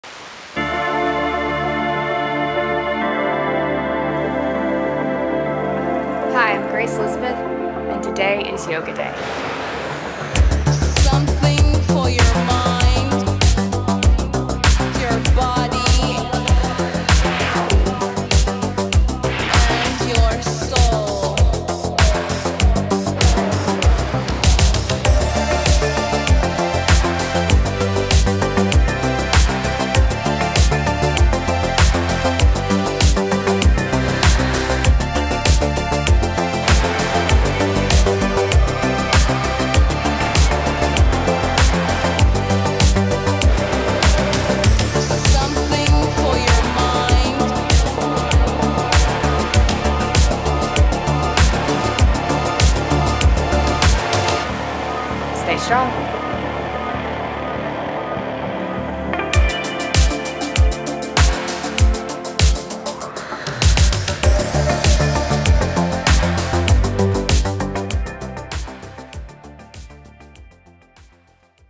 4th January | Grace Elizabeth by Phil Poynter | Love Advent 2017
**January Bonus!!**
The 4th day of January is Grace Elizabeth doing her best table top pose for yoga day LOVEADVENT. “I LOVE the Love Advent because it gives me the opportunity to be sexy and strong and just have fun!” STAYSTRONG.